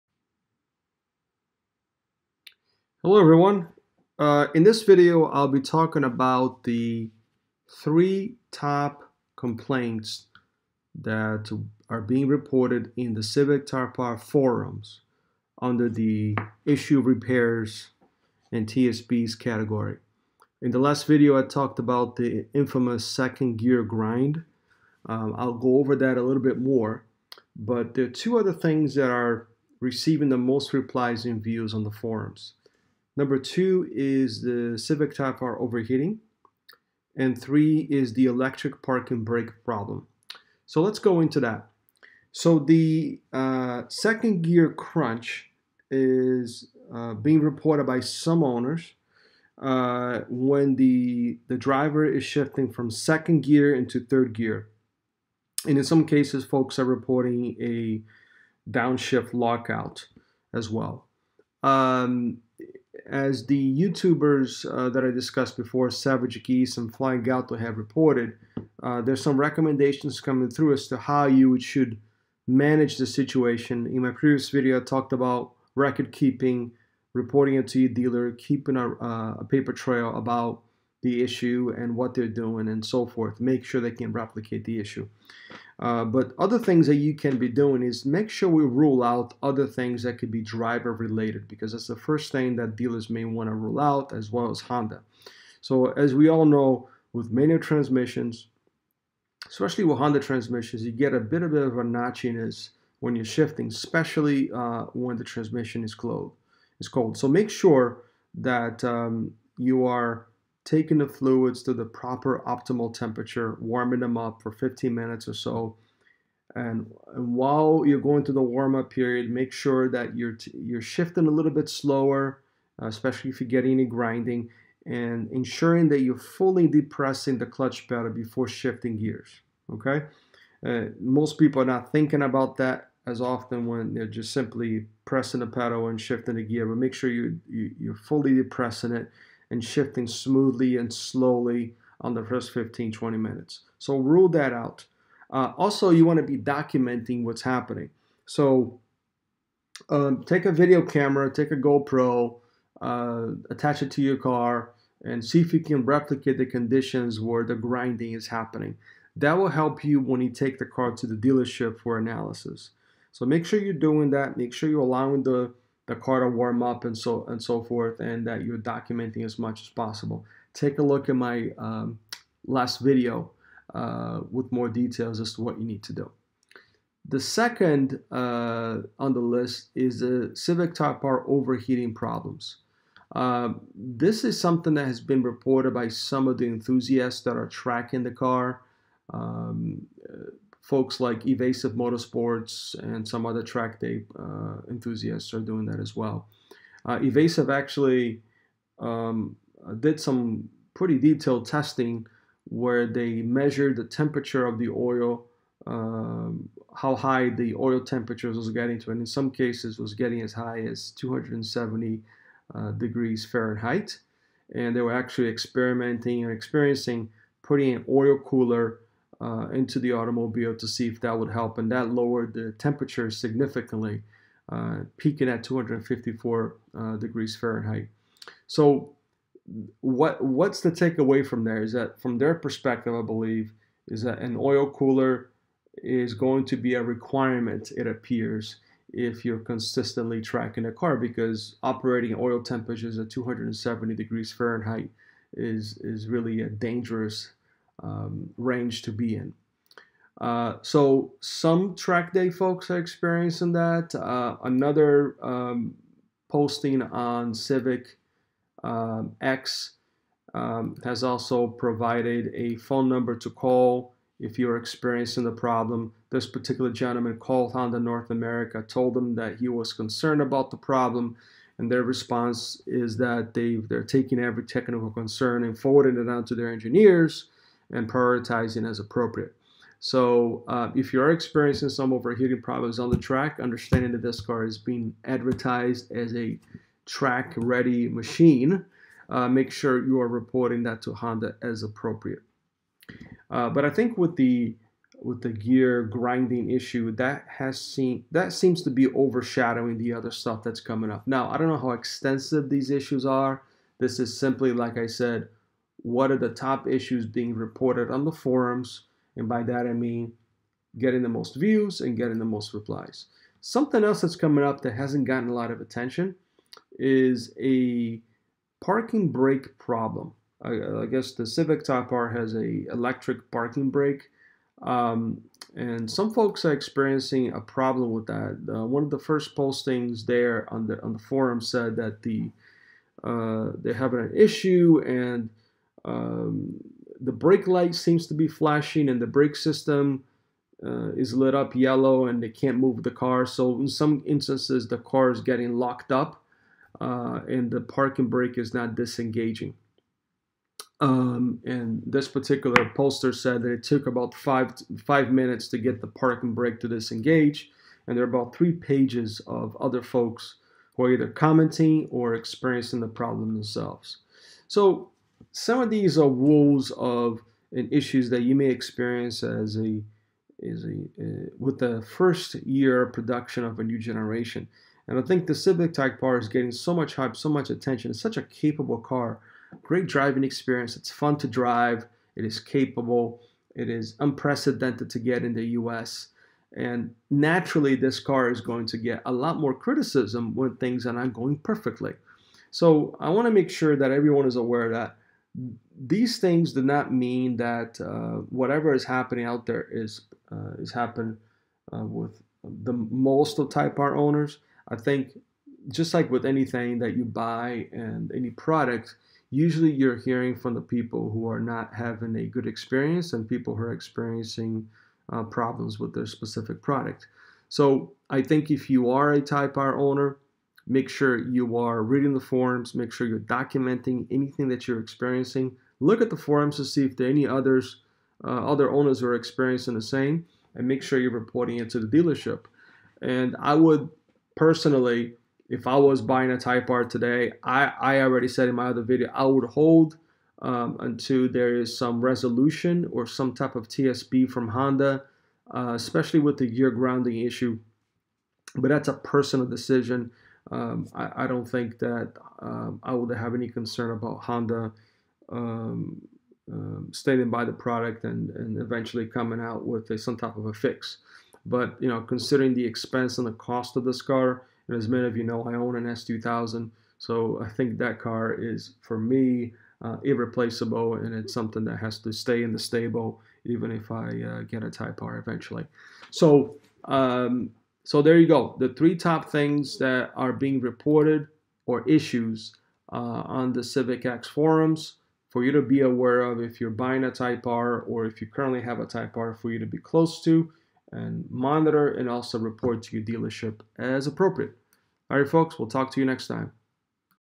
Civic Type R - Top 3 Future Tech Service Bulletins?
These are the Top 3 things current and future owners should pay attention to during ownership.